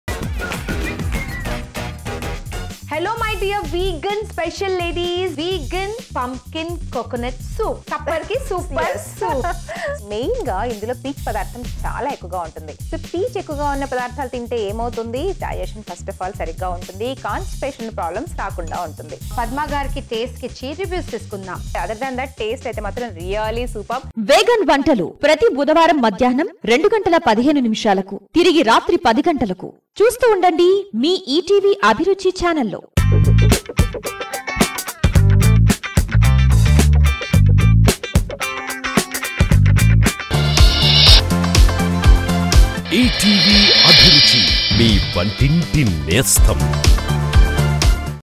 Vegan Foods | వీగన్ వంటలు | Latest Promo
Vegan Vantalu are appropriate for all stages of life. The vegan diets are plant based and animal free diets.